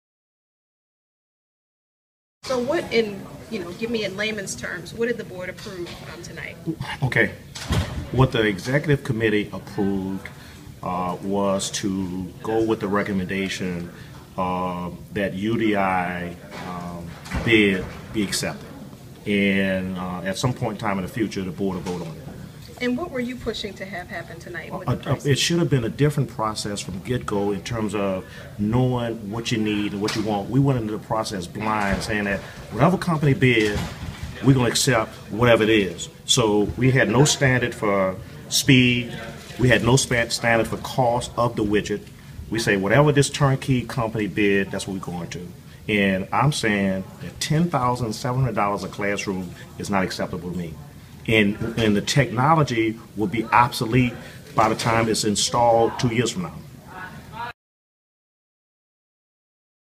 Cedric Floyd on Jefferson School Board committee meeting
Jefferson Parish School Board president Cedric Floyd discusses the board&apos;s actions in committee Tuesday (March 17.) The committee voted to spend $33 million in wireless technology upgrades for campuses.